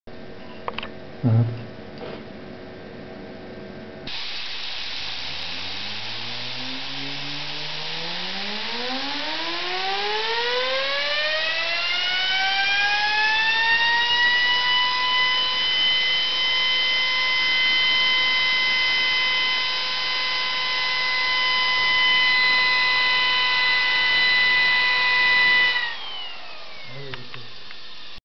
Phoenix Mk4 turbine balancing by Dynams (Original bench)
Phoenix Mk4 turbine balancing by DYNAMS software based dynamic balancing solution (Original bench setup) from HUNGARY